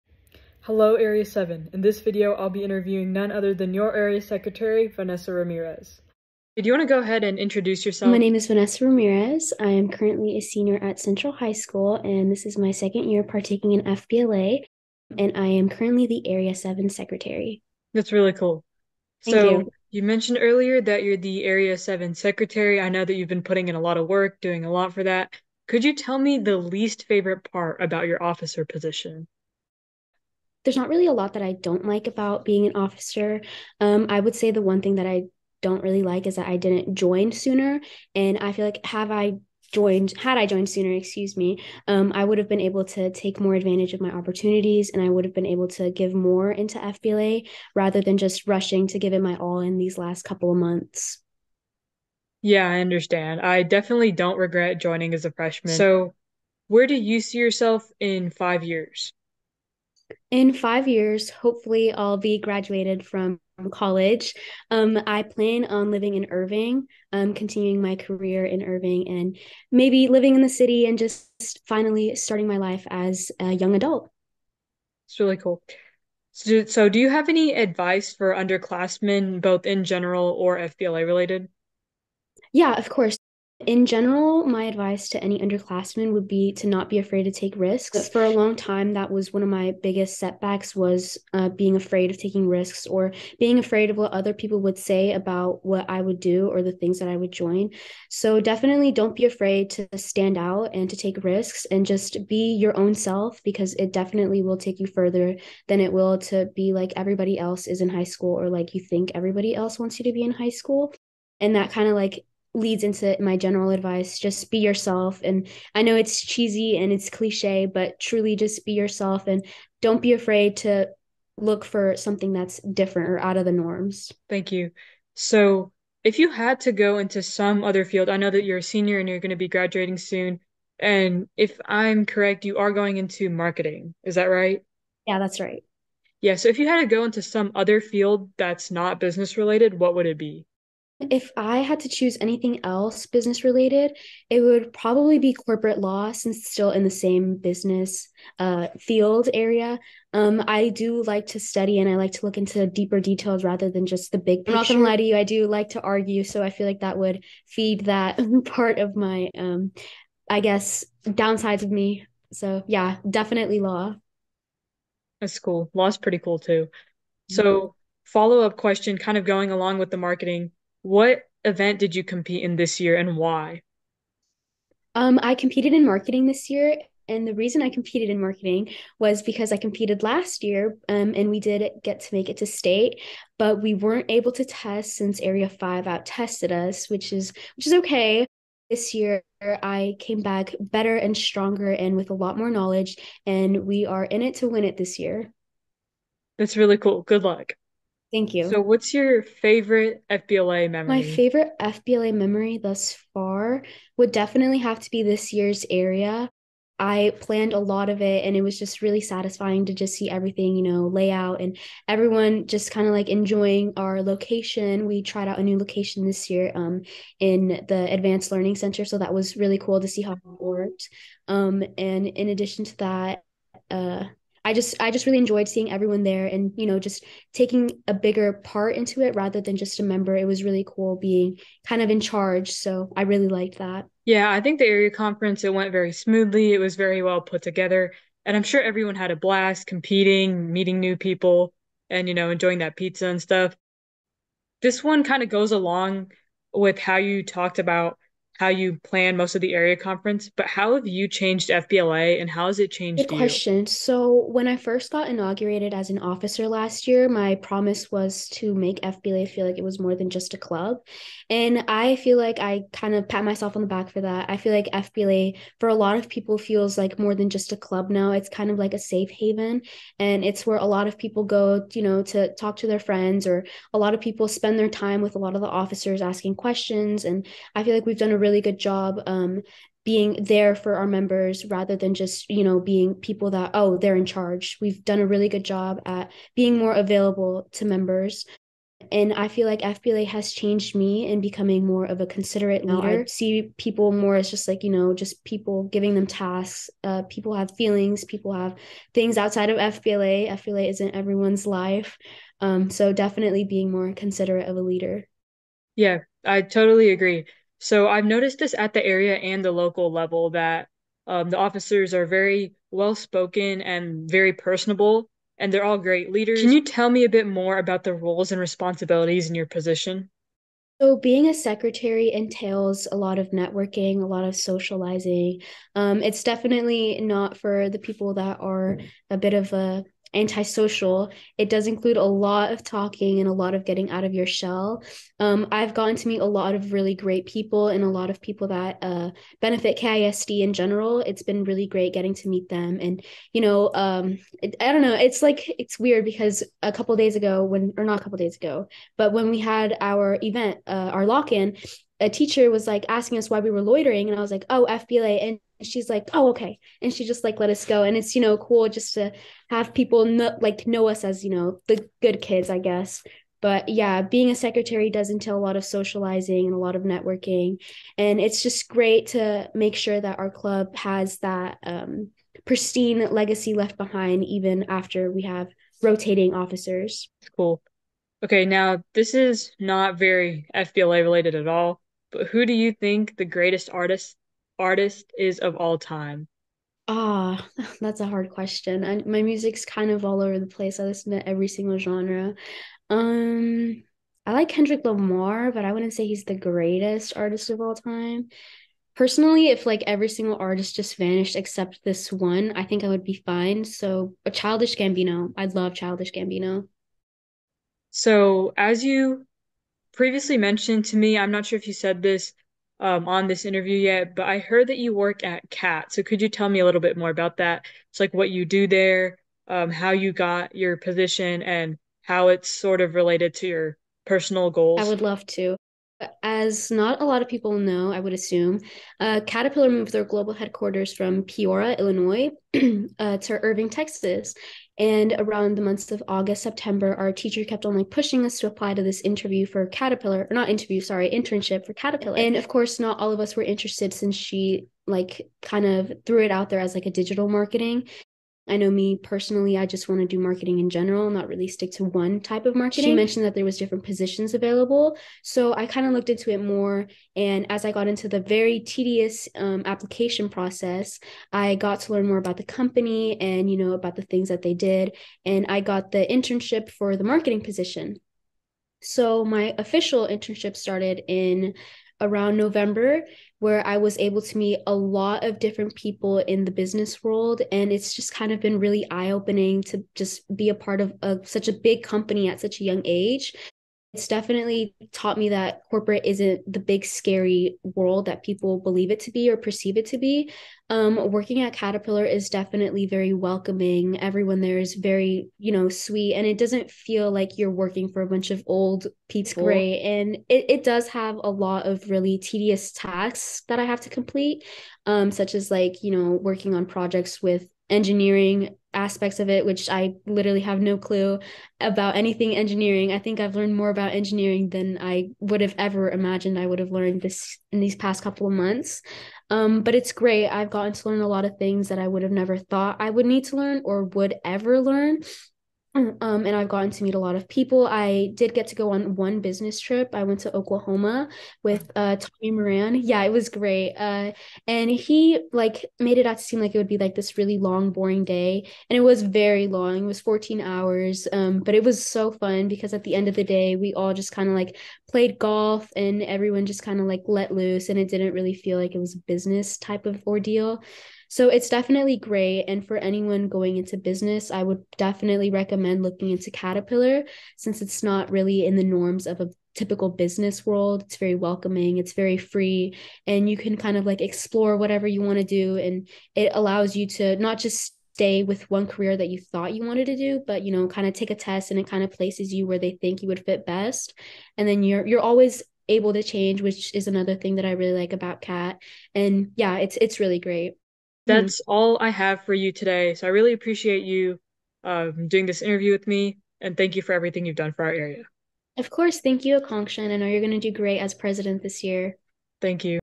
Meet Your Area 7 Secretary (2022-2023)| Ft. Vanessa Ramirez
Happy Valentine's Day! In this video, we will be interviewing Vanessa Ramirez from Central High School who is currently serving as your Area 7 Secretary. Follow along as we learn more about her goals, FBLA experience, and internship!

Thank you Vanessa for doing this interview!

Make sure you check us out on our other platforms :)